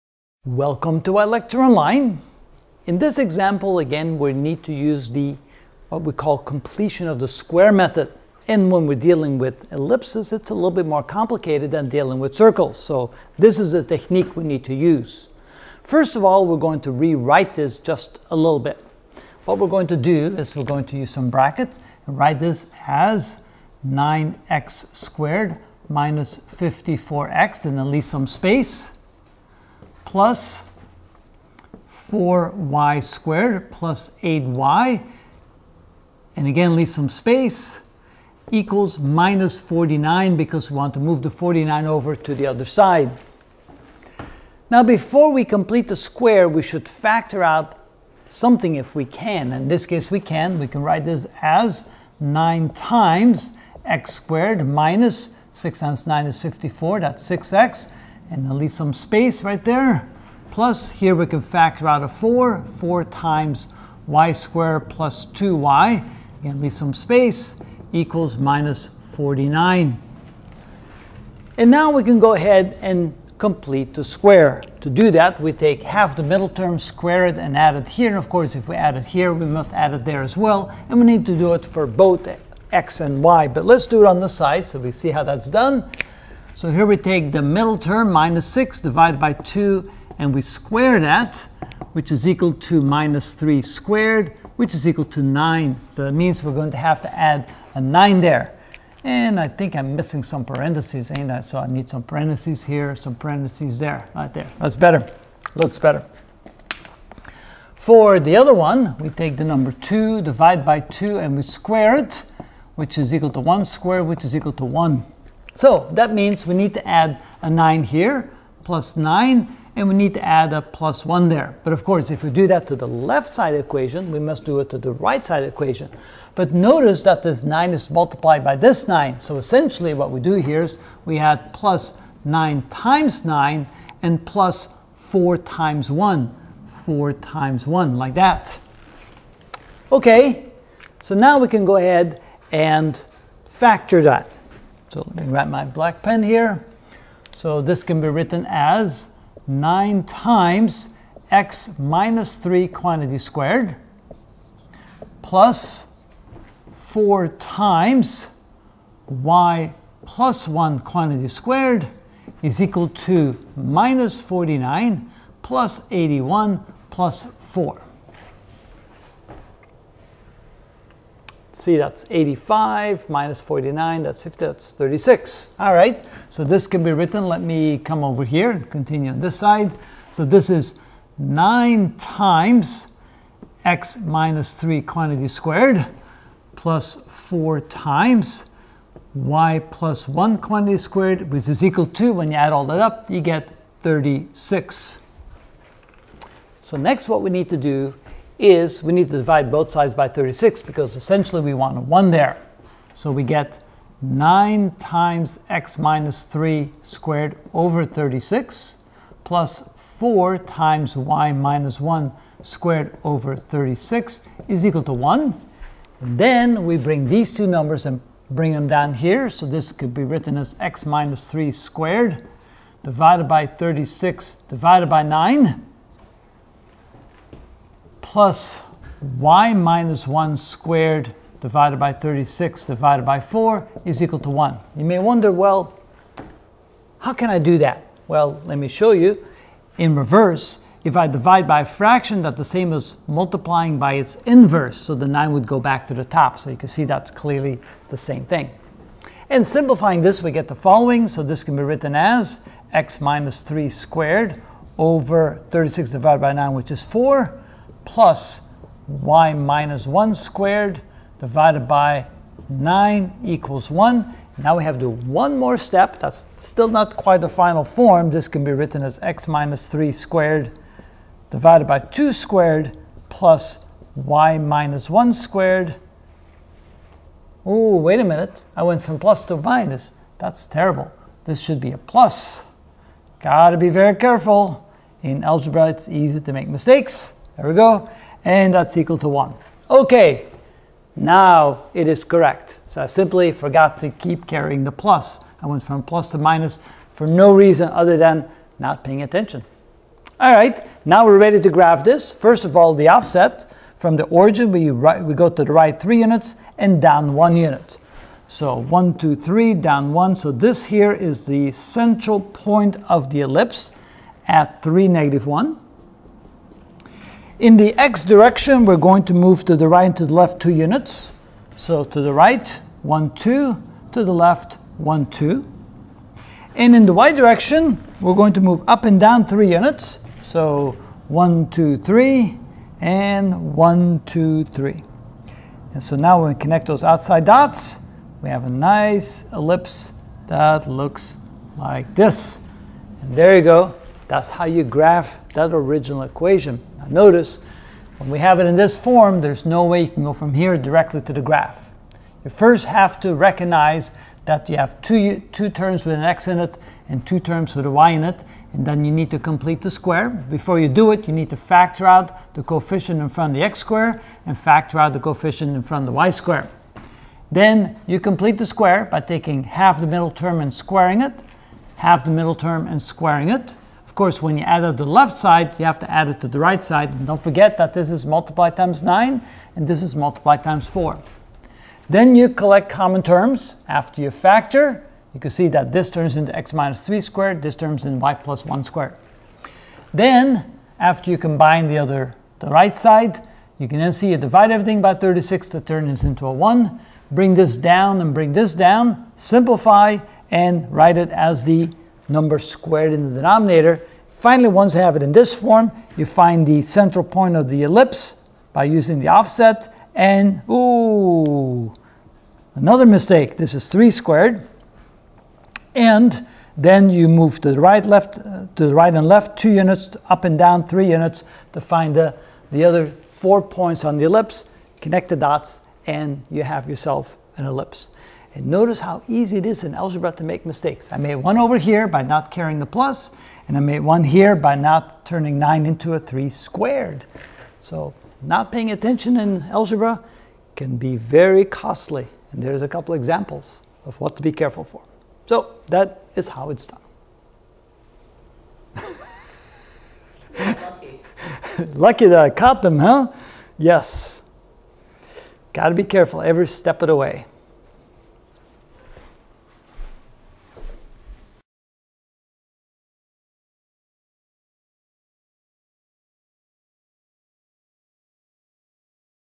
Algebra Ch 39: Ellipse (7 of 8) Graphing an Ellipse: Example 1
We will be given an equation and will need to rewrite the equation into the general form of the equation of an ellipse, and then we will graph the ellipse.
Example 1: 9x^2-54x+4y^2+8y+49=0